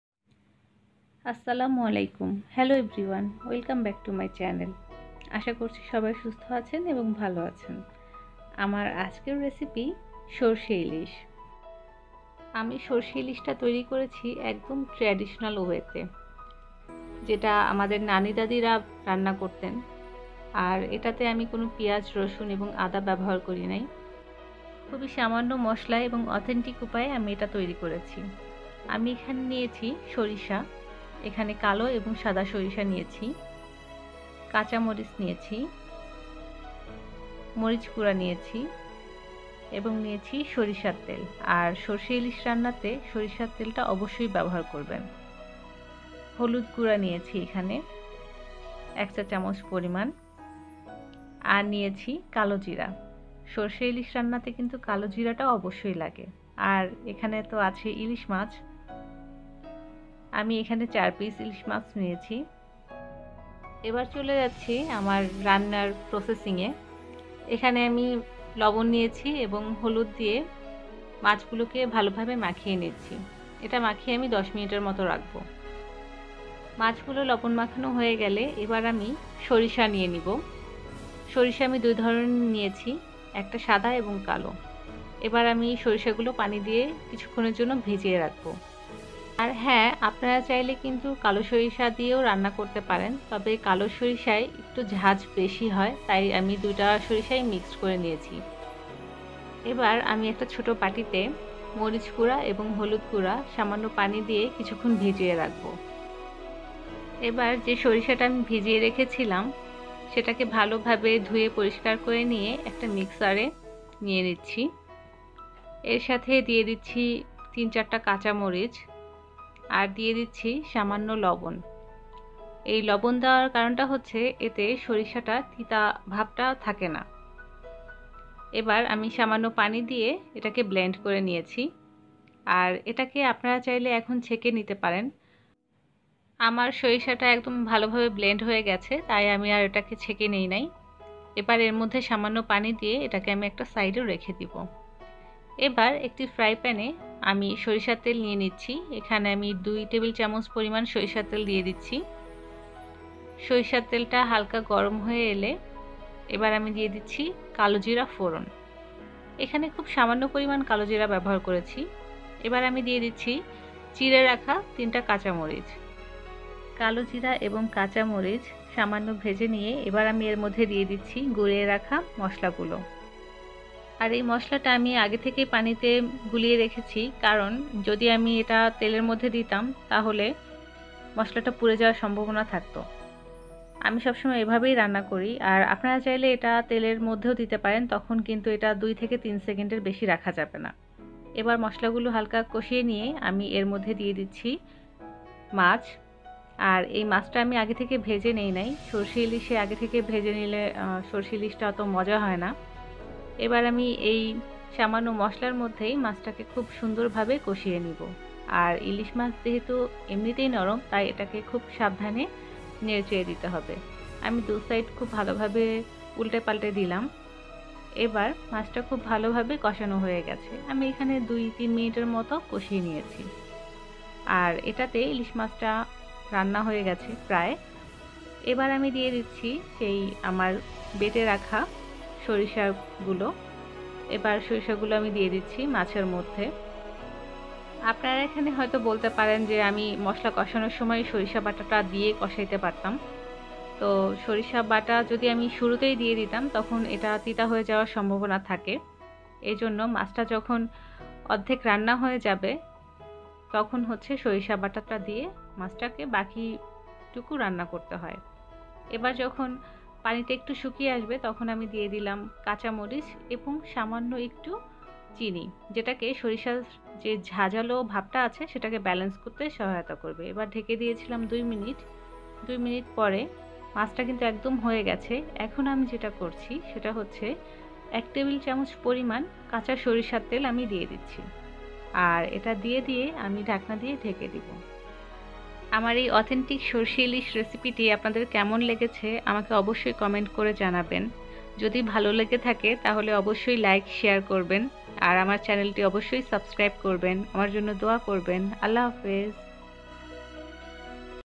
সরিষা ইলিশ মাছের রেসিপি | সর্ষে ইলিশ রান্নার রেসিপি | সবচেয়ে সহজ সরিষা ইলিশ রান্নার রেসিপি
উপকরণঃ

★ইলিশ মাছ -৪পিছ
★কাঁচা মরিচ
★সাদা সরিষা -১ টেবিল চামচ
★কালো সরিষা - ১ চা চামচ
★সরিষার তেল
★হলুদ গুঁড়া
★শুকনা মরিচ গুঁড়া
★কালোজিরা
★লবন
★চিনি